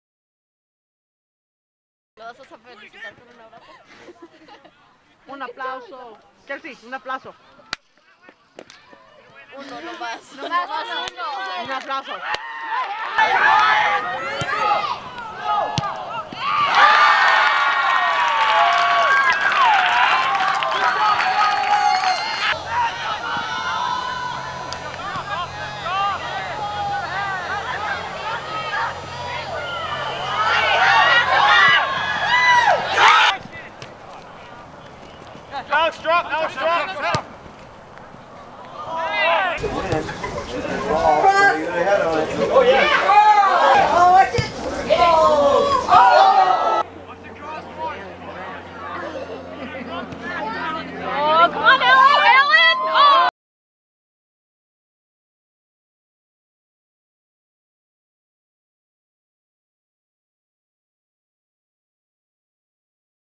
Alex White Highlights
Highlights from Alex White's senior season at Tigard High School.